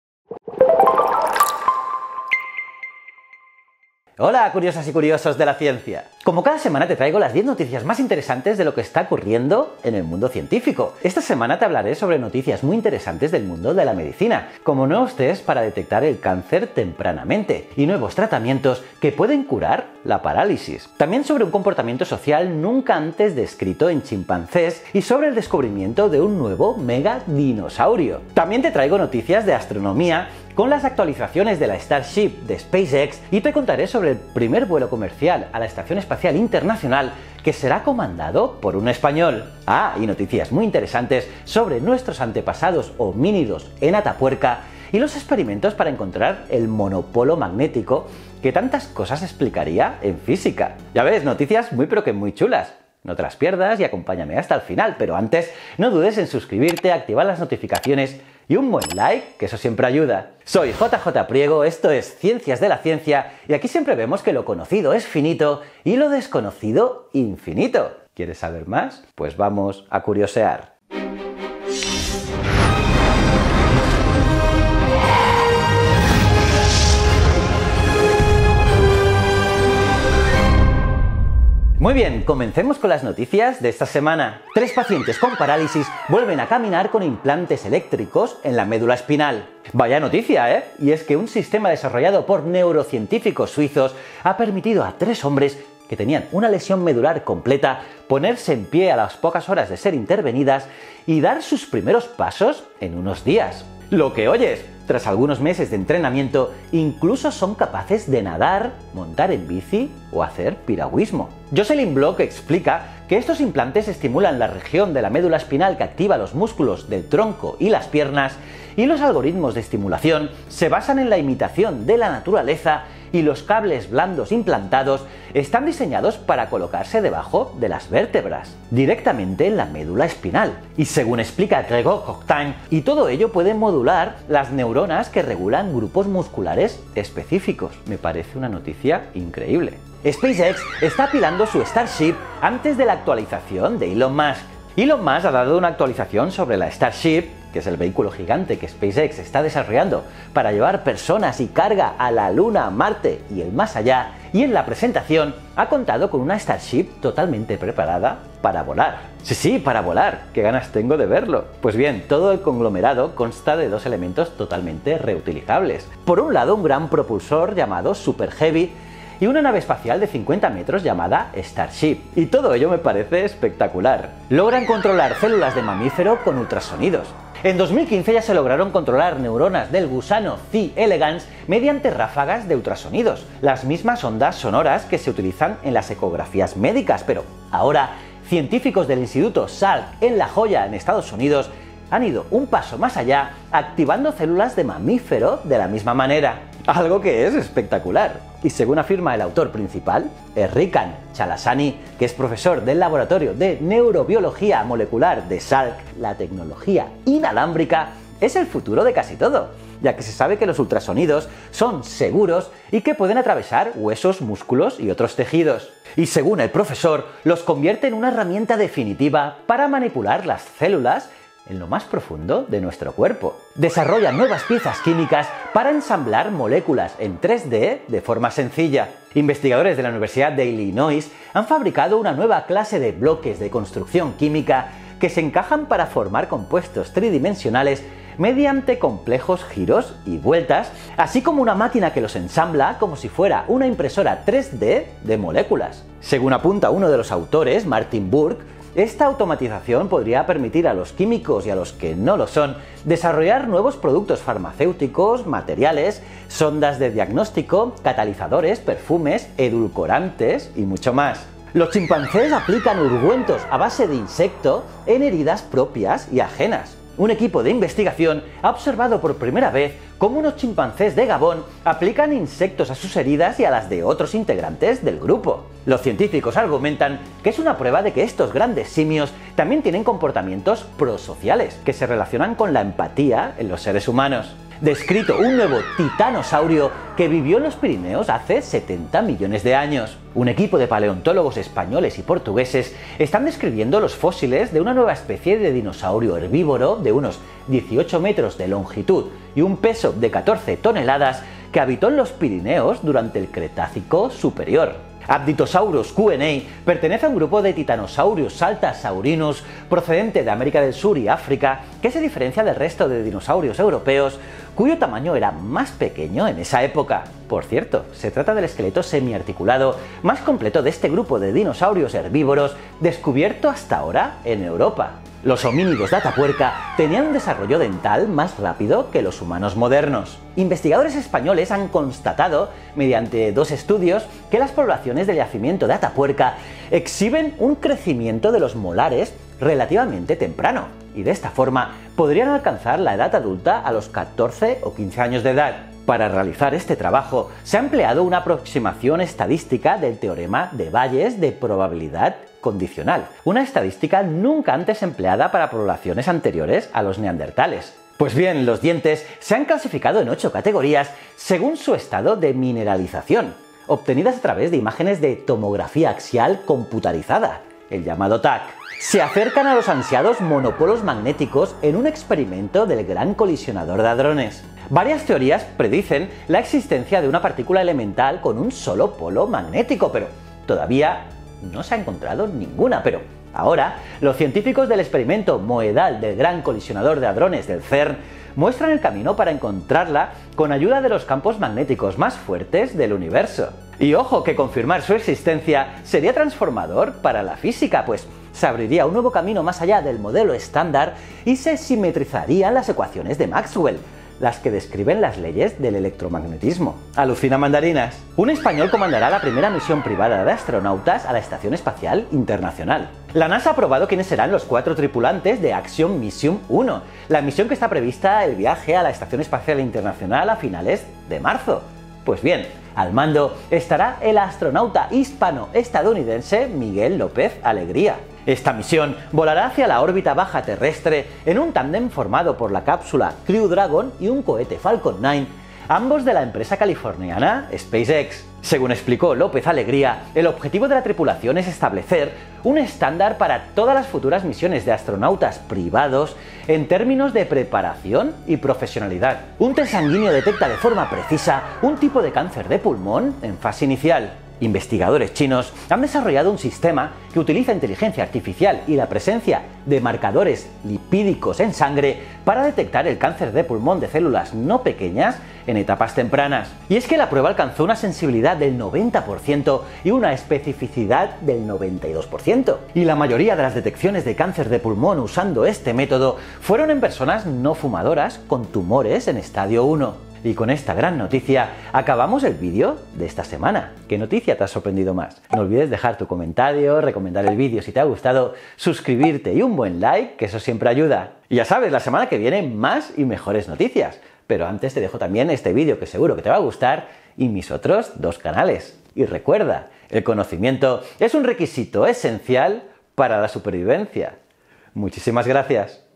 Tres pacientes con parálisis vuelven a caminar con implantes eléctricos | Noticias Científicas
Las mejores Noticias de CIENCIA de la semana!

0:00 Introducción
1:24 Tres pacientes con parálisis vuelven a caminar con implantes eléctricos en la médula espinal
2:19 SpaceX está apilando su Starship antes de la actualización de Elon Musk
2:53 Logran controlar células de mamífero con ultrasonidos
3:43 Desarrollan nuevas piezas químicas para ensamblar moléculas en 3D de forma sencilla
4:21 Los chimpancés aplican ungüentos a base de insecto en heridas propias y ajenas
4:44 Descrito un nuevo titanosaurio que vivió en los Pirineos hace 70 millones de años
5:24 Los homínidos de Atapuerca tenían un desarrollo dental más rápido que los humanos modernos
6:07 Se acercan a los ansiados monopolos magnéticos en un experimento del Gran Colisionador de Hadrones
6:47 Un español comandará la primera misión privada de astronautas a la Estación Espacial Internacional
7:29 Un test sanguíneo detecta de forma precisa un tipo de cáncer de pulmón en fase inicial

𝓜𝓾𝓬𝓱𝓪𝓼 𝓰𝓻𝓪𝓬𝓲𝓪𝓼

𝓙𝓙 😘👌